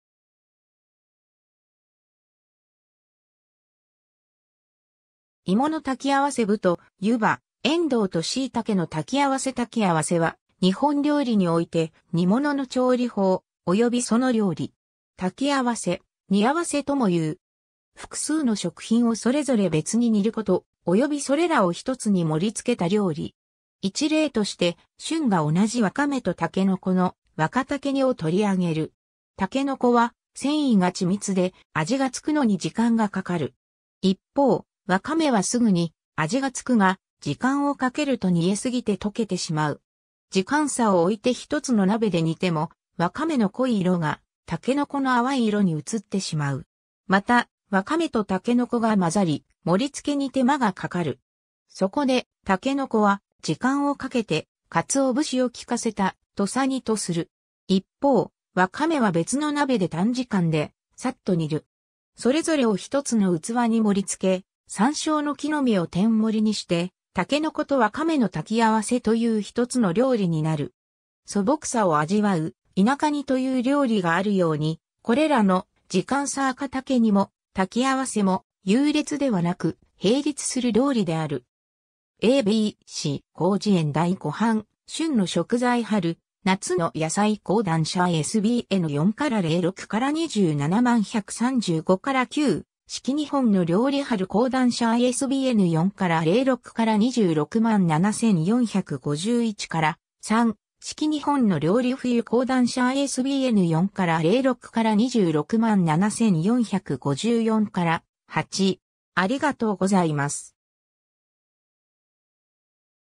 炊き合せ
芋の炊き合せ 麸と湯葉、エンドウとシイタケの炊き合せ 炊き合せ（たきあわせ）は日本料理において煮物の調理法、およびその料理。
「焚き合せ」「煮合わせ」ともいう。
複数の食品をそれぞれ別に煮ること、およびそれらを1つに盛り付けた料理。
 一例として、旬が同じワカメとタケノコの「若竹煮（わかたけに）」をとりあげる。
 タケノコは繊維が緻密で、味が付くのに時間がかかる。
一方、ワカメはすぐに味が付くが、時間をかけると煮えすぎて溶けてしまう。
時間差をおいて1つの鍋で煮ても、ワカメの濃い色がタケノコの淡い色に移ってしまう。
また、ワカメとタケノコが混ざり、盛り付けに手間がかかる。
 そこで、タケノコは時間をかけて鰹節を効かせた「土佐煮」とする。
一方、ワカメは別の鍋で短時間でさっと煮る。
それぞれを1つの器に盛り付け、サンショウの木の芽を天盛りにして、「筍とわかめの炊き合せ」という1つの料理になる。
 素朴さを味わう「田舎煮」という料理があるように、これらの「時間差若竹煮」も「炊き合せ」も優劣ではなく、並立する料理である。
 ^ a b c 広辞苑 第5版 ^ 『旬の食材 春・夏の野菜』講談社 ISBN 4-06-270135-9 ^ 『四季日本の料理 春』講談社 ISBN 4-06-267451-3 ^ 『四季日本の料理 冬』講談社 ISBN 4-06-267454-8